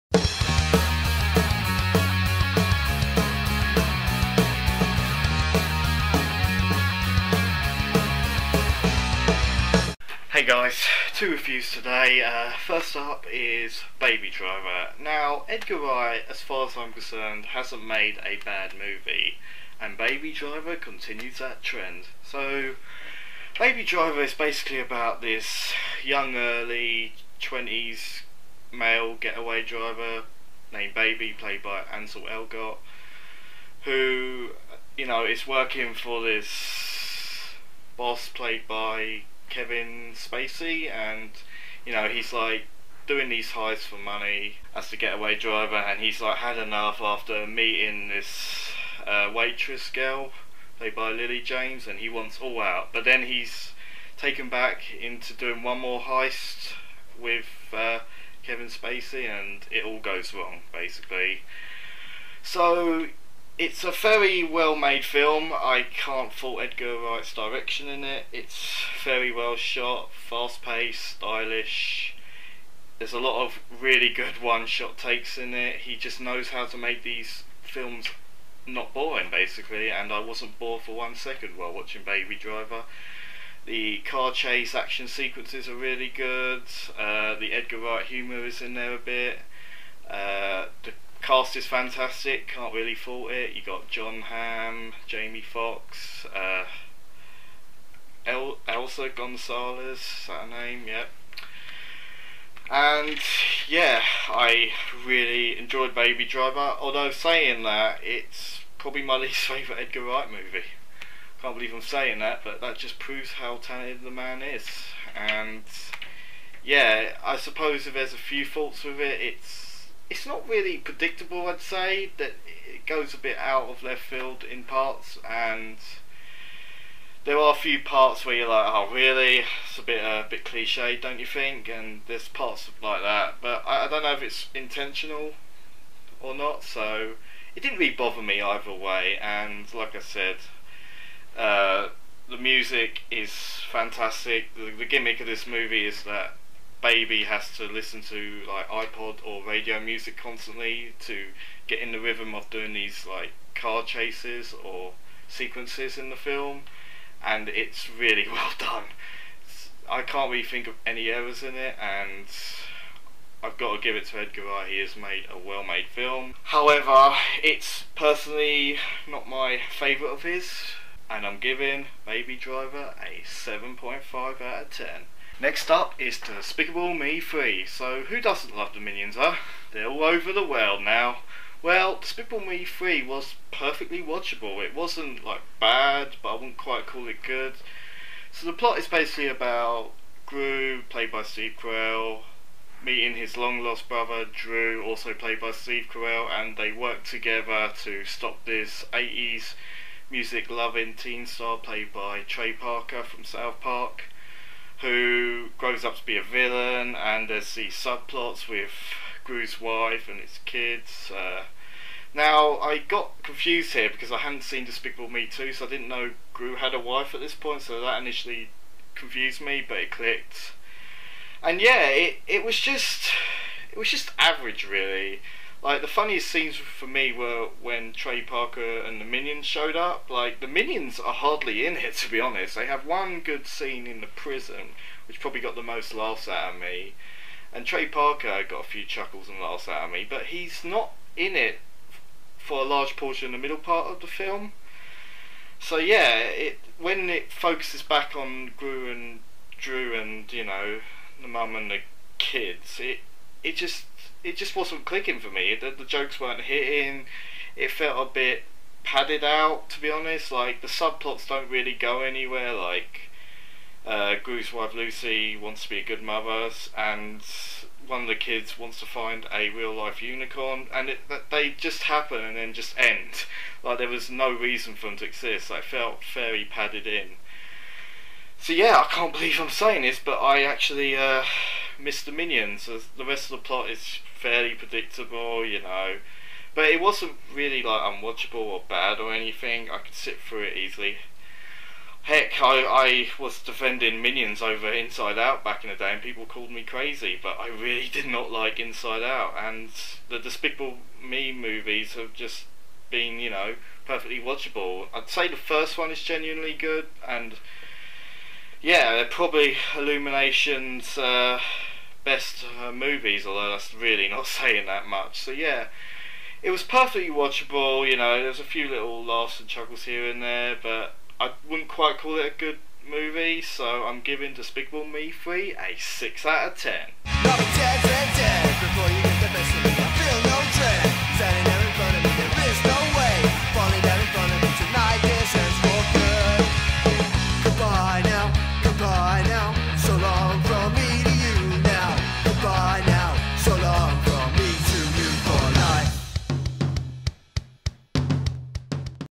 Baby Driver/Despicable Me 3 Movie Review
My movie review of Baby Driver and Despicable Me 3. As much as I'd like to use movie clips, I don't want to have to deal with that Copyright crap. Hopefully, just using images will be alright.